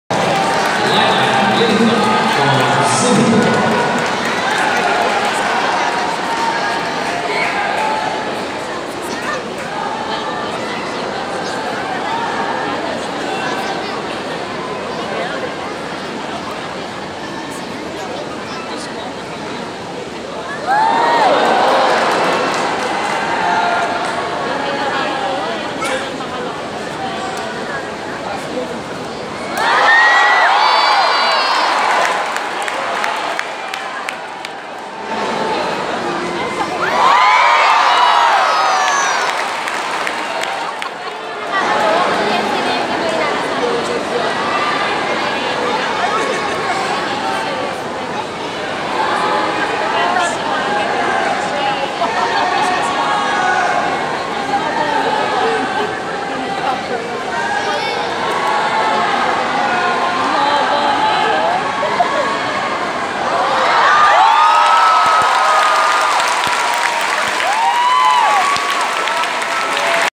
Singaporean gymnast Liqht Man Lincoln - Horizontal bars
The tallest among the gymnasts, Lincoln Liqht Man does a solid horizontal bar routine.